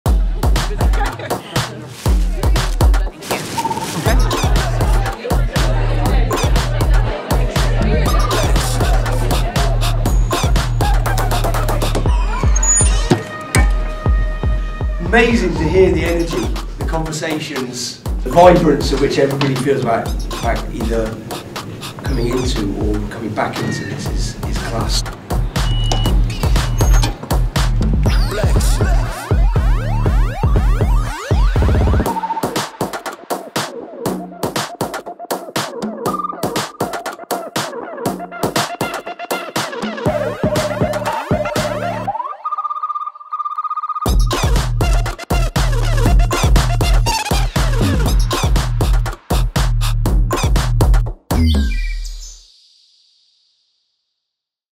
Inside Camp: Ireland Women's Squad Assemble
It was the first day of camp for the Ireland squad as they prepare for the Guinness Women's Six Nations.